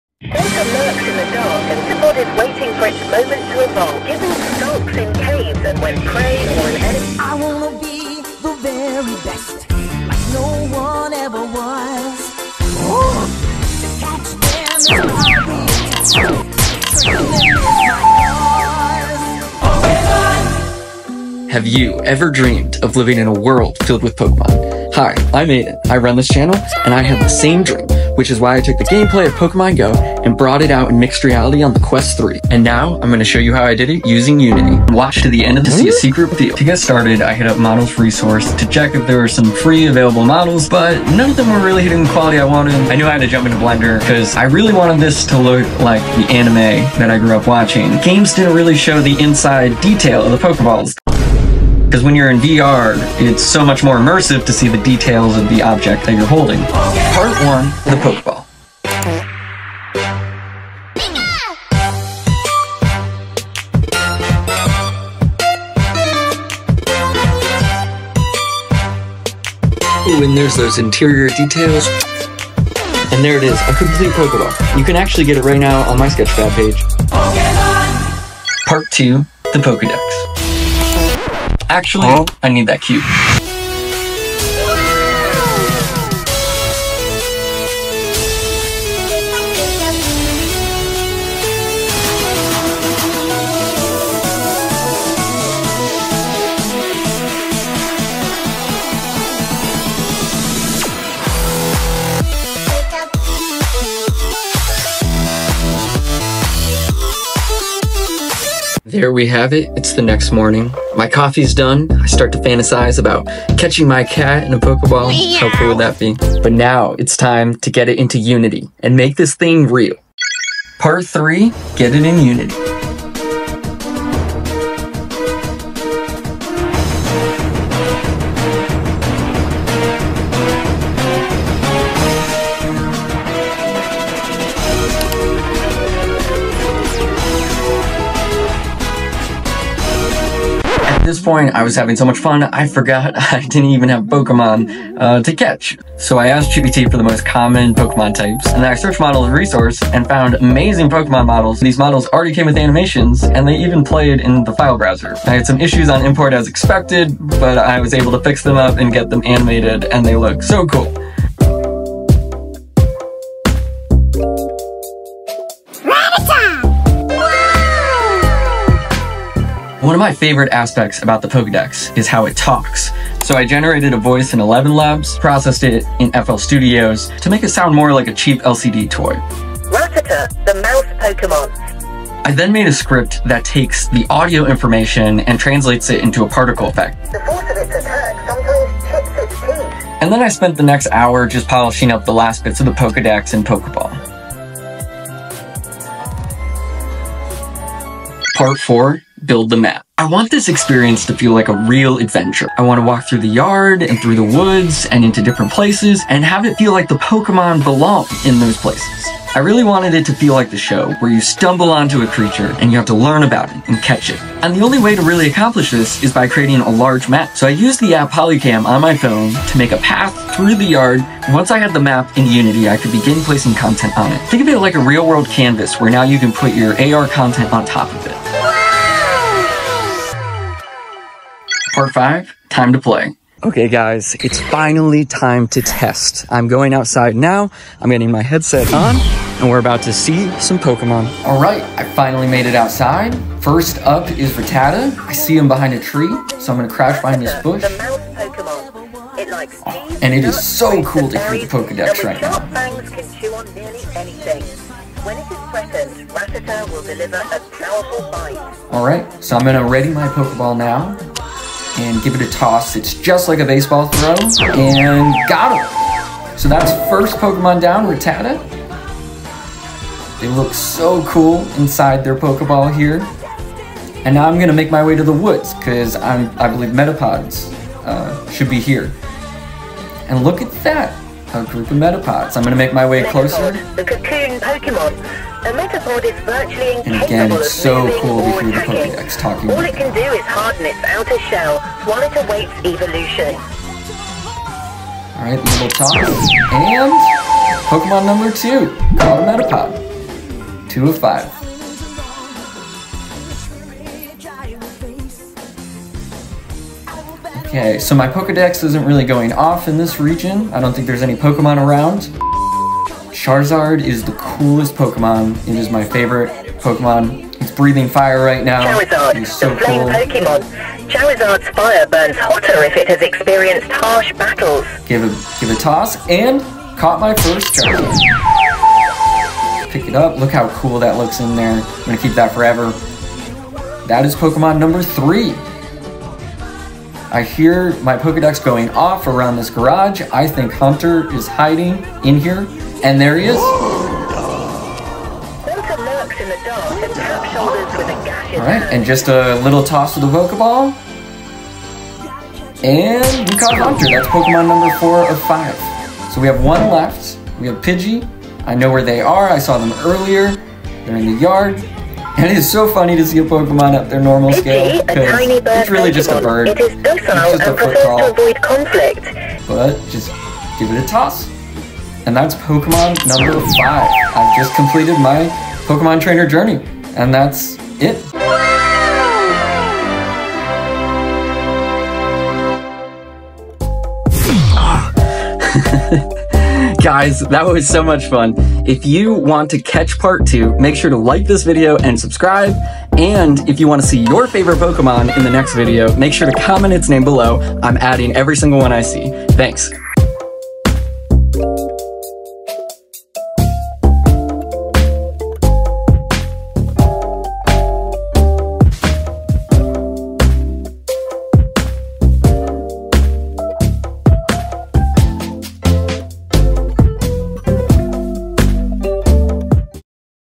I Remade Pokémon in Real Life! Quest 3 Mixed Reality Devlog
In this video I take on the ultimate challenge of re-creating Pokémon in real life using the power of Quest 3 Mixed Reality, Blender, and Unity! From concept to code I’ll walk you through the entire development process and take you on a real life Pokémon adventure fans could have only dream of going on previously!

0:00 Introduction
0:55 Making the Pokéball
1:18 Making the Pokédex
2:00 Making the Game (Unity)
3:18 Making the Map
3:50 Play time!!
4:18 Catching Rattata
4:50 Catching Metapod
5:18 Catching ████
5:50 Catching Haunter
6:10 Catching Pidgey
6:18 Closing Thoughts

This project is intended for educational and entertainment purposes only, showcasing the possibilities of mixed reality technology using Quest 3 and Unity. No commercial use of Pokémon intellectual property is intended or implied.